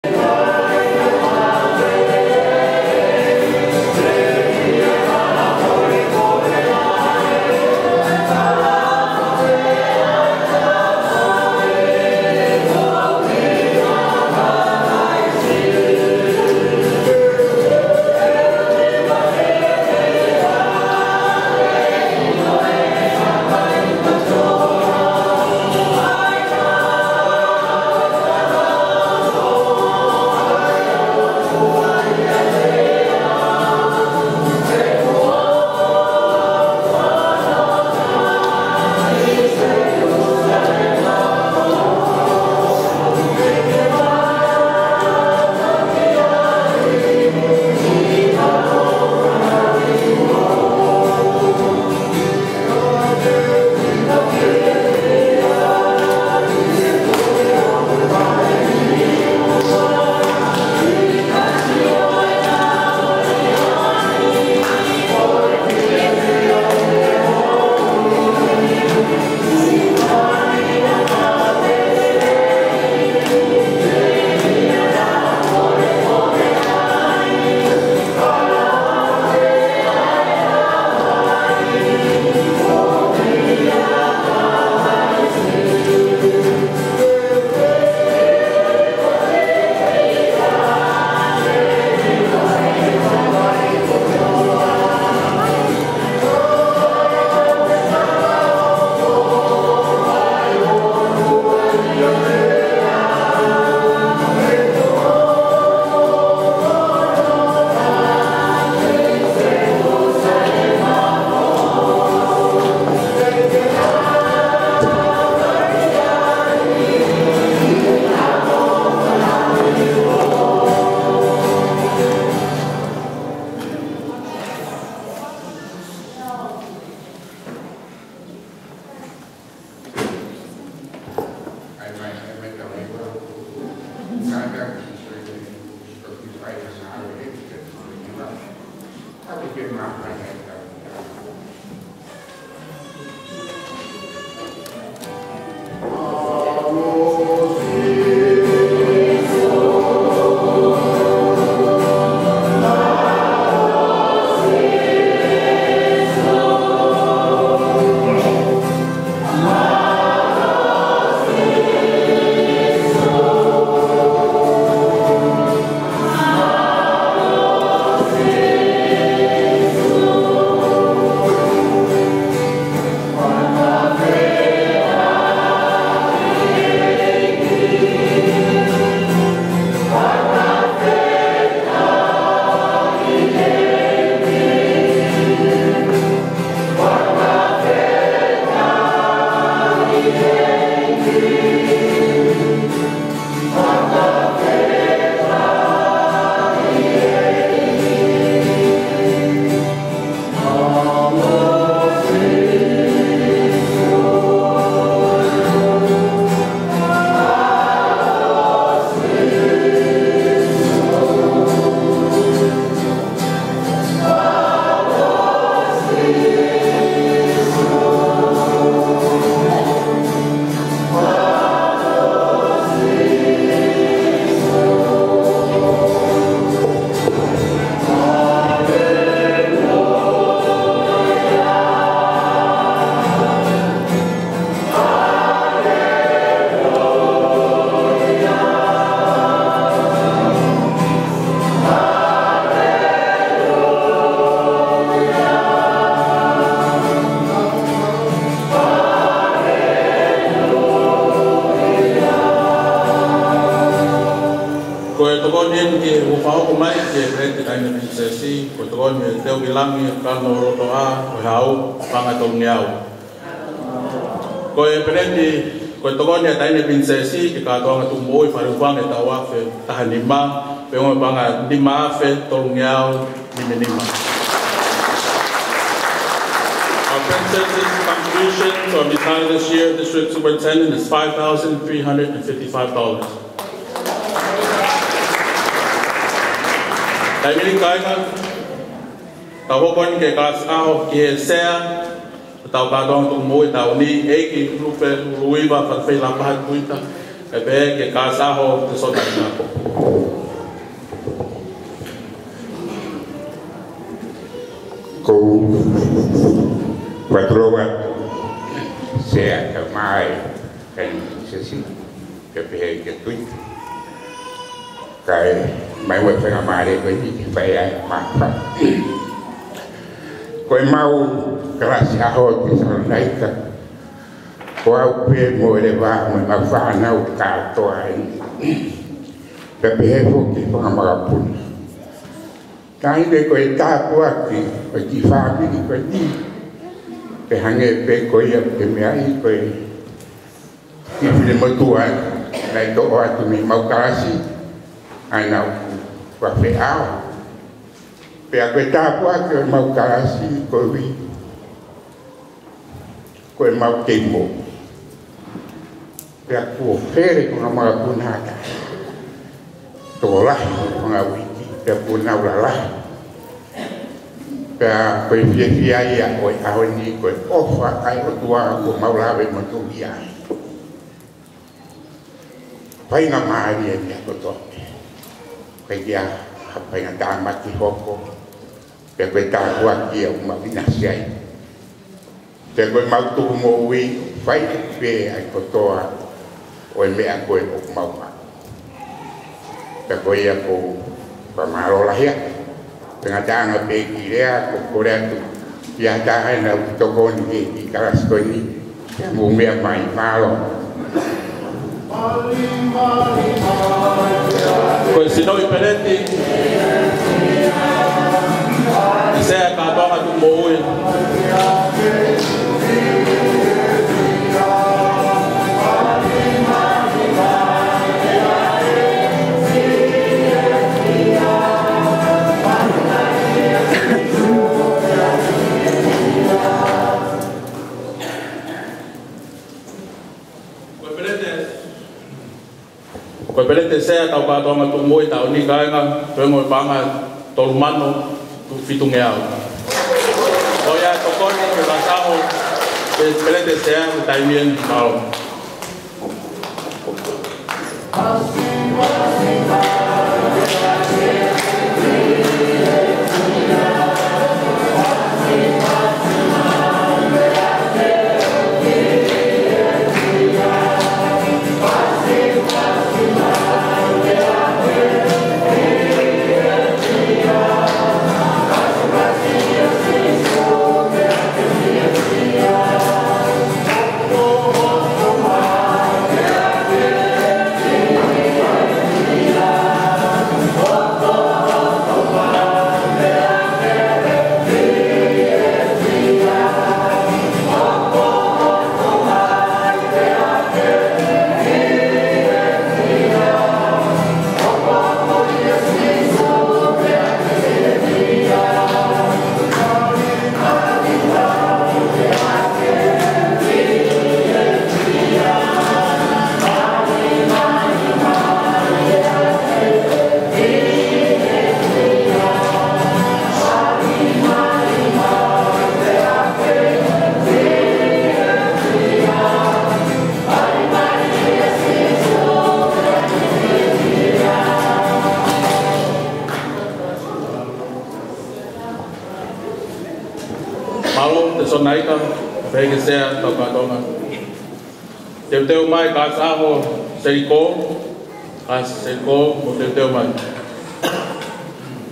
MC/SIONE MOALA JR. ~ KALASI 'AHO 'A TESOLONAIKA
2015 FALEHUFANGA UMC KATOANGA TUKUMO'UI/21 NOV. 2015.  Sea 'oe Katoanga ~ Hon. Frederica Lupe'uluiva Fatafehi 'o Lapaha Tuita Filipe.  & Produced for Global Vision One TV by Siosifa 'Isama'u & God Bless.